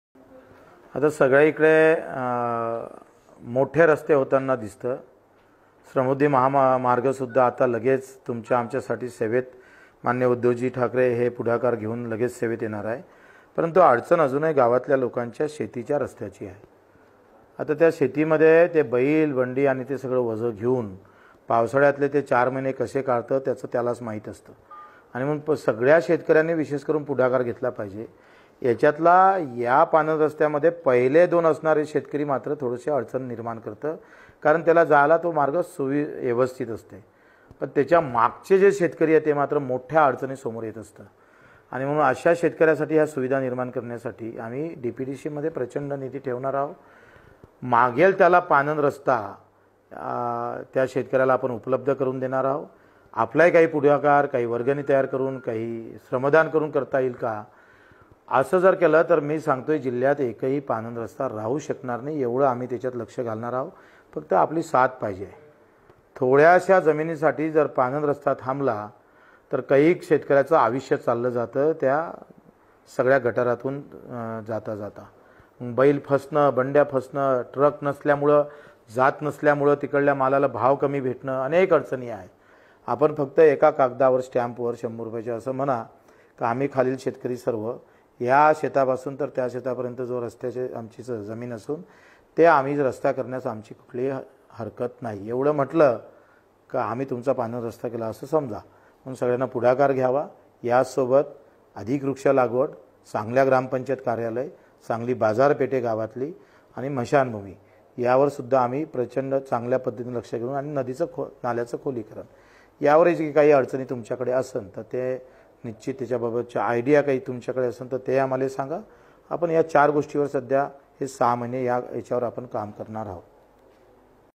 गावातील रस्त्यावर मा. बच्चू कडू यांचे महत्वाचे निर्णय।। मागेल त्याला पांधन रस्ता मिळणार।।
Maharashtra state minister bachhu kadu bhau latest updates speech
Bacchu kadu bhau